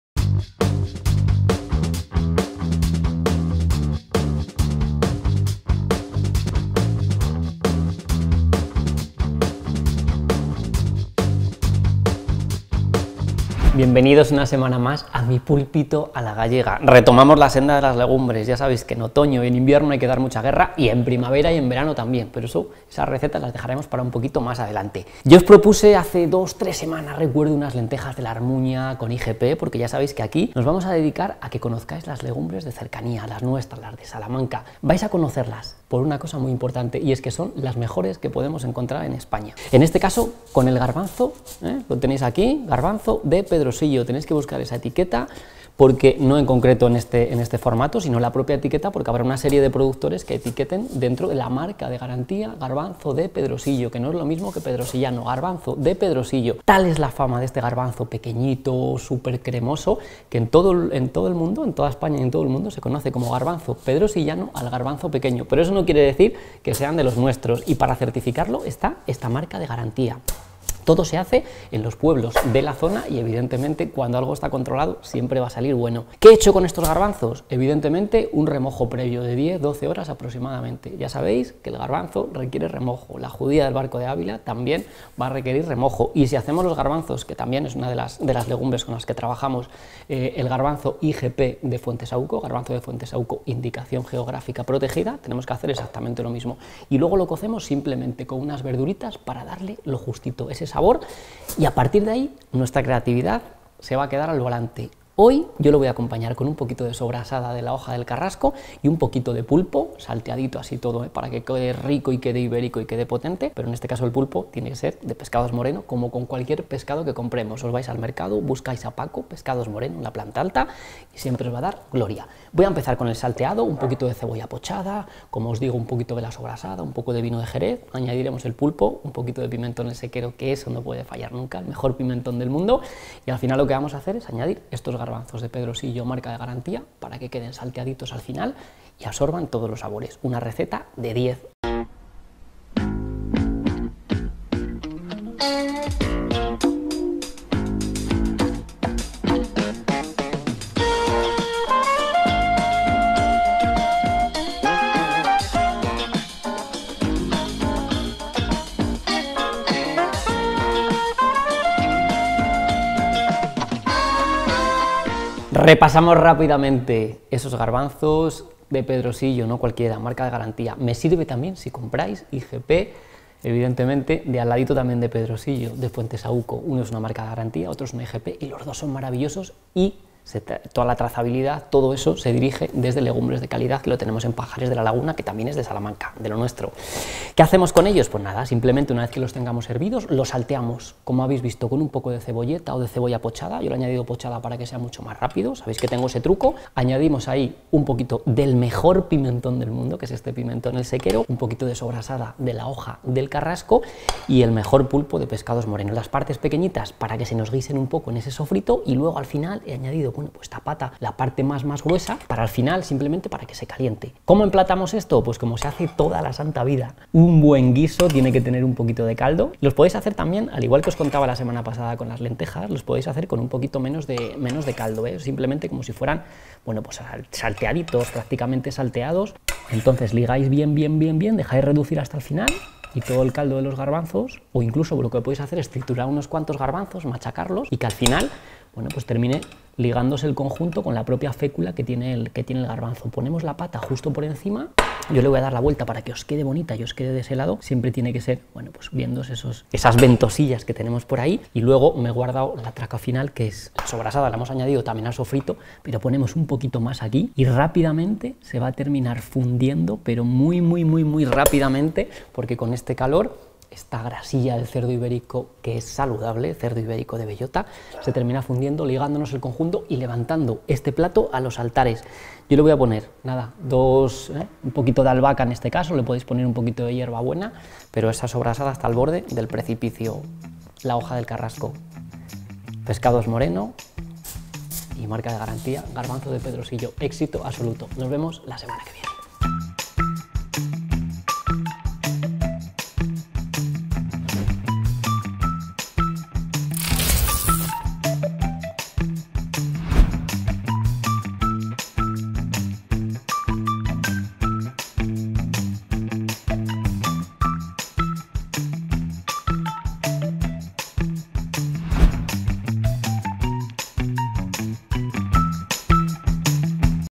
Garbanzos de Pedrosillo Marca de Garantía salteados con pulpo y sobrasada
Salteado de garbanzos de Pedrosillo Marca de Garantía con Pulpo y sobrasada. Receta fácil, sencilla y sabrosa elaborada por David Monaguillo.